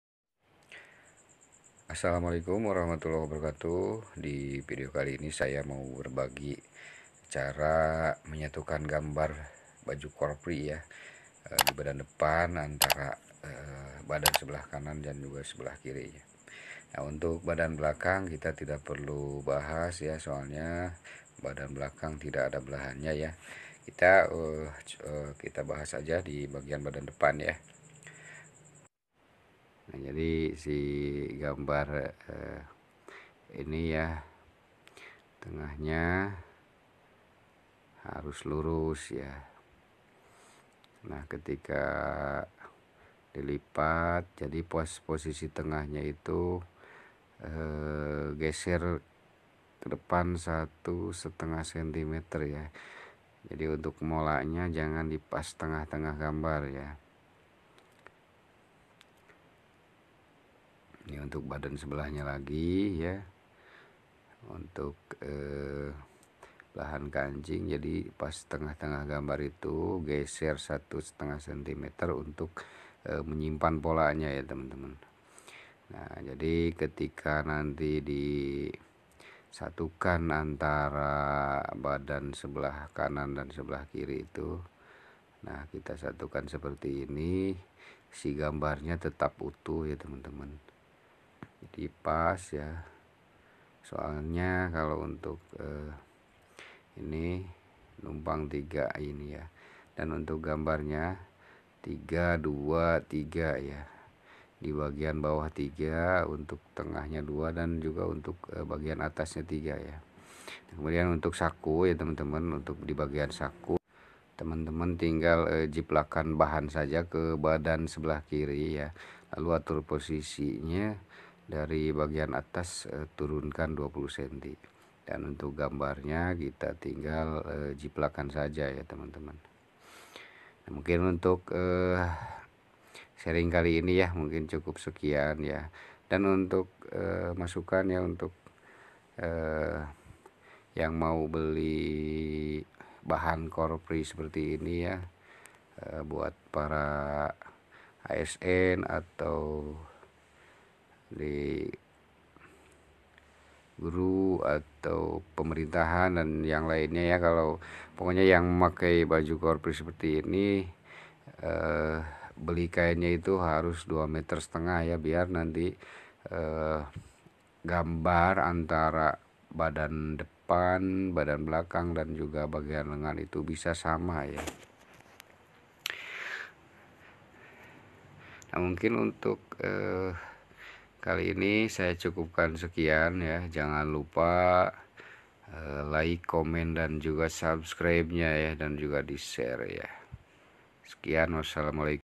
cara menyambungkan gambar di baju korpri dan posisi gambar yang benar 323
cara menyambungkan gambar di baju korpri dan posisi gambar yang benar
tips:
untuk yang mau beli bahan baju korpri ukuran nya harus 2,5 M,biar gambar antara badan depan baju,belakang,lengan dan bagian yang lain nya bisa nyeri,323
sebetul nya 2m pun bisa jadi,cuman gambar nya tidak akan sama/nyeri antara bdan depan sama belakang d posisi 323